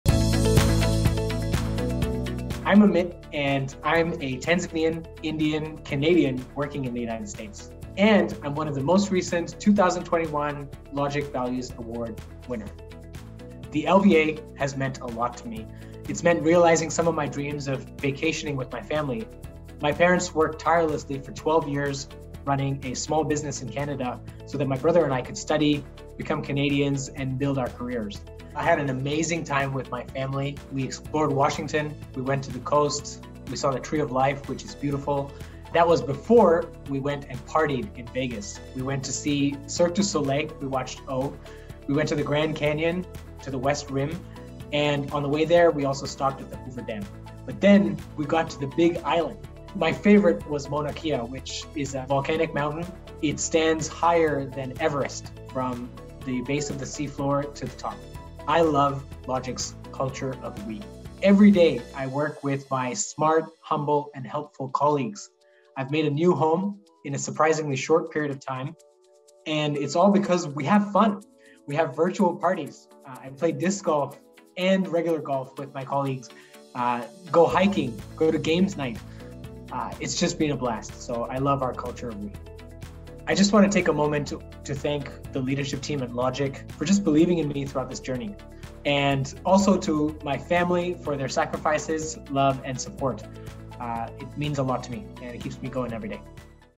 2021 Logical Values Award - Amit U!
We're proud to announce one of our winners of the Logical Values Award, which honors an employees who lives our values day in and day out. Congratulations to Amit Unadkat! He was named a winner in 2021.

Watch his video here to learn more about his trip exploring various cities with his family around the U.S. He also shares more about why he loves working at Logic20/20. We're so thankful to have Amit on our team and for everything he does for us!